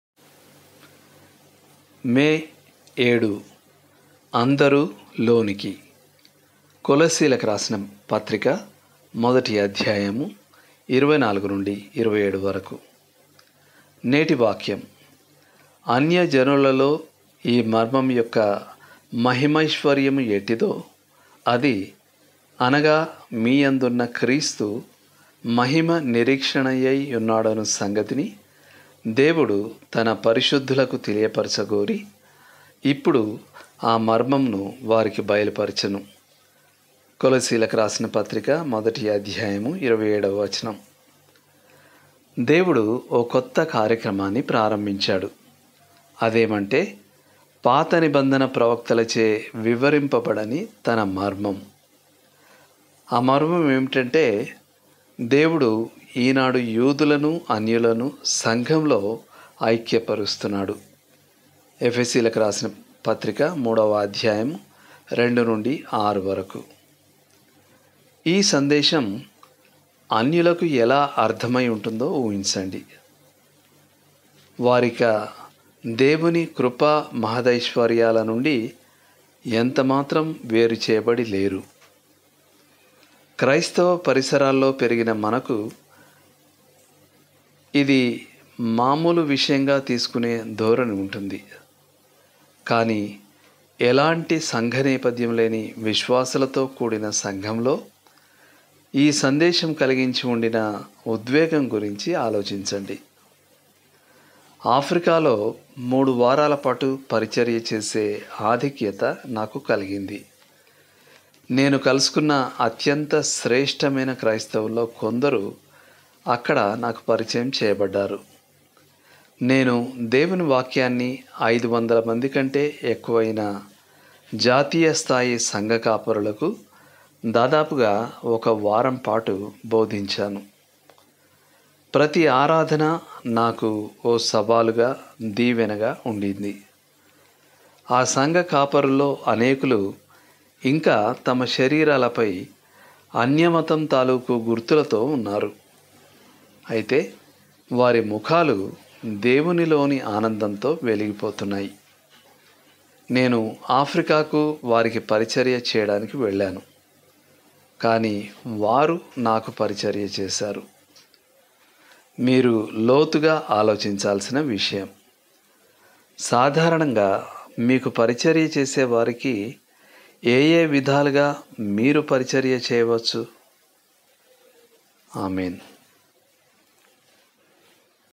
Telugu | May 7 | Pause for Power | Warren W Wiersbe
7 మే 2023   అందరూ  లోనికి

చదవండి:  కొలస్సీ 1:24-27

నేటి వాక్యం: అన్యజనులలో ఈ మర్మముయొక్క మహిమైశ్వర్యము ఎట్టిదో అది, అనగా మీయందున్న క్రీస్తు, మహిమ నిరీక్షణయై యున్నాడను సంగతిని దేవుడు తన పరిశుద్ధులకు తెలియపరచగోరి యిప్పుడు ఆ మర్మమును వారికి బయలుపరచెను(కొలస్సీ. 1:27).

దేవుడు ఓ కొత్త కార్యక్రమాన్ని ప్రారంభించాడు - అదేమంటే, పాత నిబంధన ప్రవక్తలచే వివరింపబడని తన మర్మం. ఆ మర్మమేమిటంటే దేవుడు ఈనాడు యూదులనూ, అన్యులనూ సంఘములో ఐక్య పరుస్తున్నాడు. (ఎఫెసీ. 3:2-6). ఈ సందేశం అన్యులకు ఎలా అర్థమై ఉంటుందో ఊహించండి. వారిక దేవుని కృపా మహాదైశ్వర్యాల నుండి ఎంత మాత్రం వేరు చేయబడి లేరు!

క్రైస్తవ పరిసరాల్లో పెరిగిన మనకు ఇది మామూలు విషయంగా తీసుకునే ధోరణి ఉంటుంది. కానీ ఎలాటి సంఘ నేపథ్యం లేని విశ్వాసులతో కూడిన సంఘంలో ఈ సందేశం కలిగించి ఉండిన ఉద్వేగం గురించి ఆలోచించండి.

ఆఫ్రికాలో మూడు వారాల పాటు పరిచర్య చేసే ఆధిక్యత నాకు కలిగింది, నేను కలుసుకున్న అత్యంత శ్రేష్టమైన క్రైస్తవుల్లో కొందరు అక్కడ నాకు పరిచయం చేయబడ్డారు. నేను దేవుని వాక్యాన్ని ఐదువందల మంది కంటే ఎక్కువైన జాతీయ స్థాయి సంఘకాపరులకు దాదాపుగా ఒక వారం పాటు బోదించాను. ప్రతి ఆరాధన నాకు ఓ సవాలుగా, దీవెనగా ఉండింది. ఆ సంఘ కాపరుల్లో అనేకులు ఇంకా తమ శరీరాలపై అన్యమతం తాలూకు గుర్తులతో ఉన్నారు, అయితే వారి ముఖాలు దేవునిలోని ఆనందంతో వెలిగిపోతున్నాయి. నేను ఆఫ్రికాకు వారికి పరిచర్య చేయడానికి వెళ్ళాను, కానీ వారు నాకు పరిచర్య చేశారు!

మీరు లోతుగా ఆలోచించాల్సిన విషయం:

సాధారణంగా మీకు పరిచర్య చేసే వారికి ఏయే విధాలుగా మీరు పరిచర్య చేయవచ్చు?